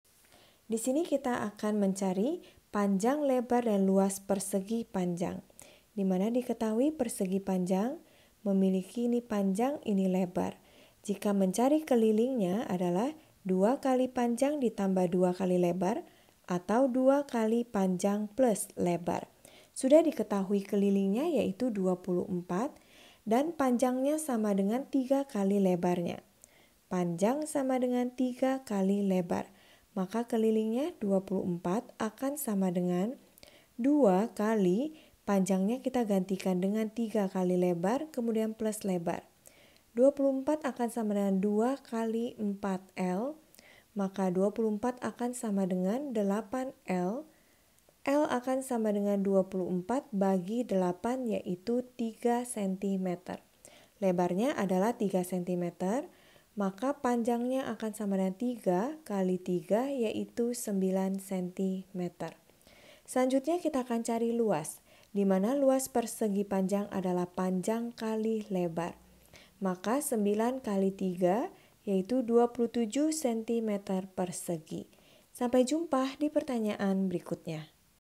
Keliling suatu persegipanjang adalah 24 cm dan panjangnya sama dengan tiga kali lebarnya, Tentuka...
Keliling suatu persegipanjang adalah 24 cm dan panjangnya sama dengan tiga kali lebarnya, Tentukan panjang; lebar; dan luas persegipanjang tersebutl

Topik atau Materi: Keliling dan Luas Persegi Panjang dan Persegi - SEGI EMPAT - GEOMETRI - Matematika Kelas 7 SMP - K-13 Revisi

Sumber Soal: Matematika Kelas 7, K-13 Revisi, SS, 2019 , Yrama Widya

00:00 : Bedah Soal
01:00 : Konsep, rumus dan pengertian Keliling dan Luas Persegi Panjang dan Persegi
01:18 : Langkah penyelesaian soal
00:18 : Jawaban akhir
00:48 : Penutup